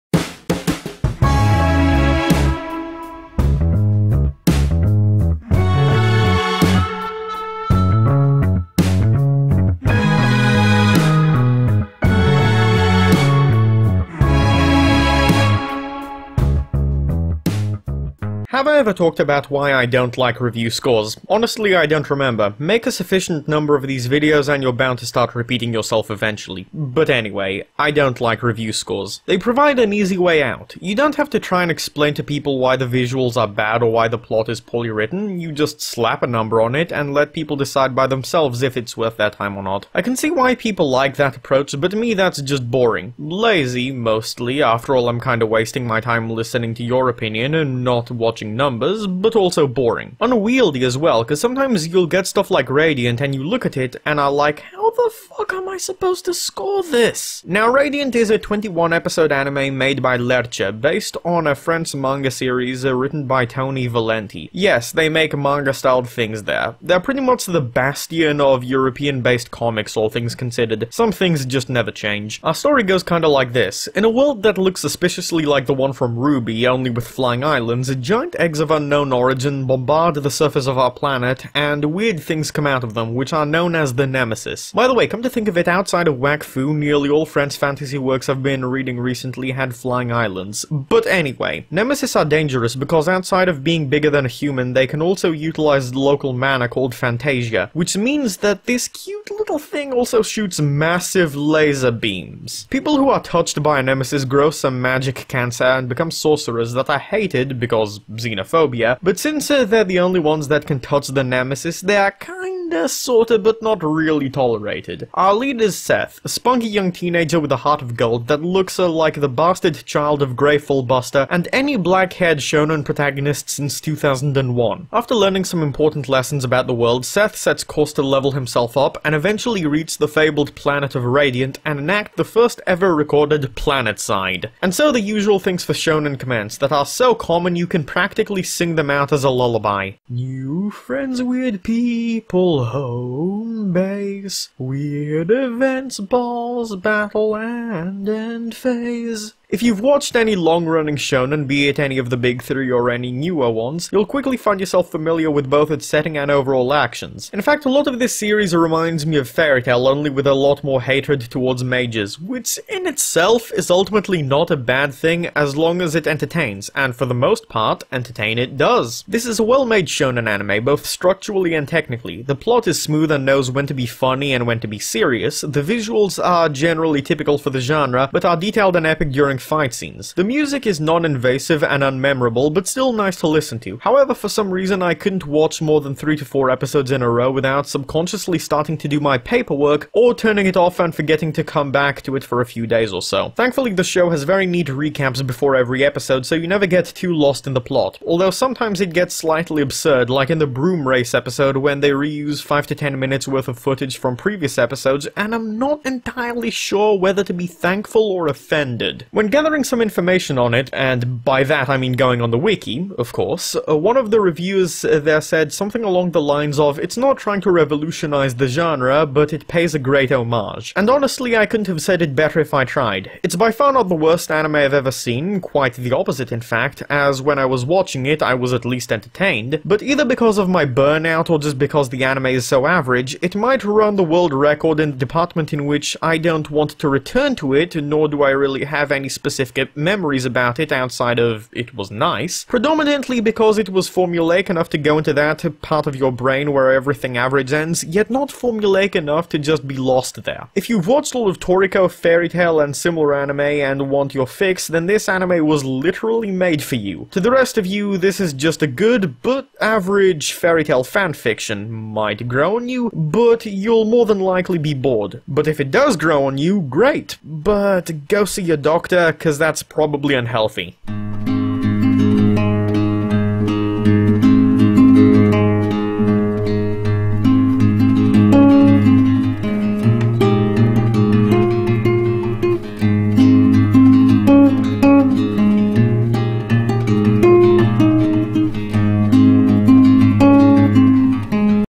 Anti rambles about Radiant
This week, Anti rambles about Radiant

I actually haz a ton of stuff, holy shit!

Картка ПБ - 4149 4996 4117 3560